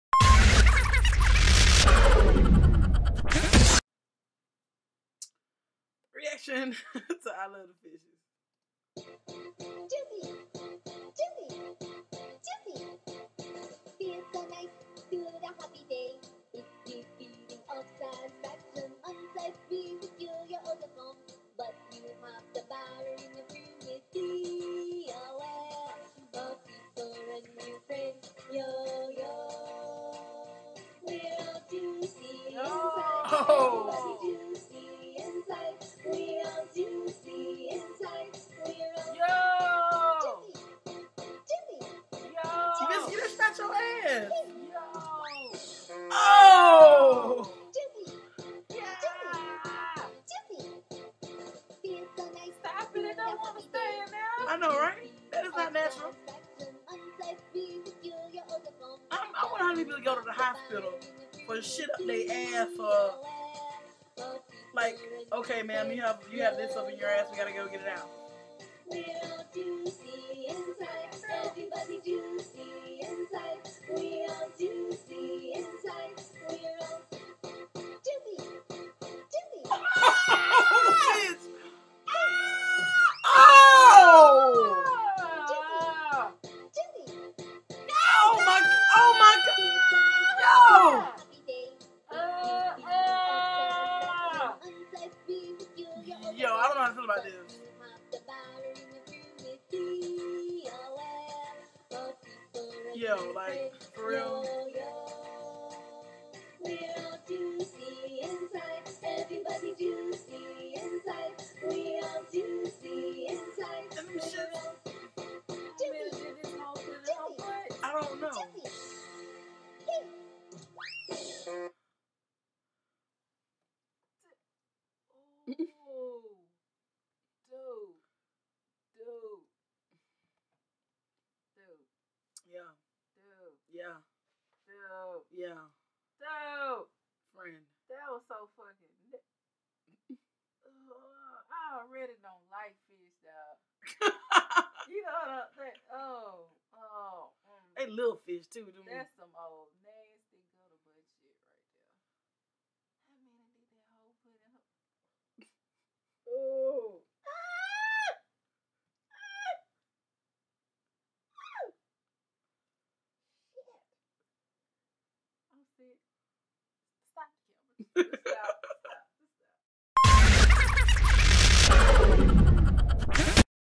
I love the fishes REACTION WITH HYP AND QCB
ME and my bestfriend's reaction to that crazy ass i love the fishes